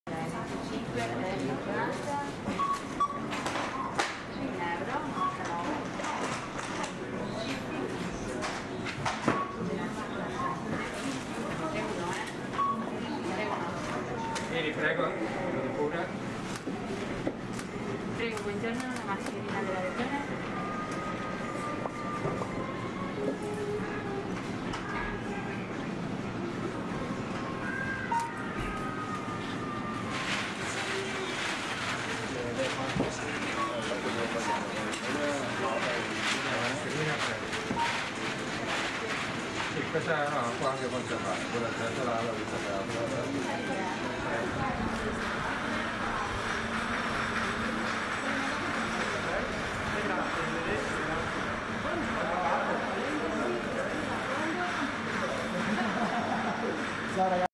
CoronaVirus (COVID-19) In Padua
The Venetian Graphic Company with the patronage of Luca Zaia, Governor of Veneto, has produced and distributed free of charge washable and reusable FFP2 masks throughout the Veneto region using supermarket chains as a means of distribution, in Padua, on March 25, 2020(Footage by Roberto Silvino/NurPhoto)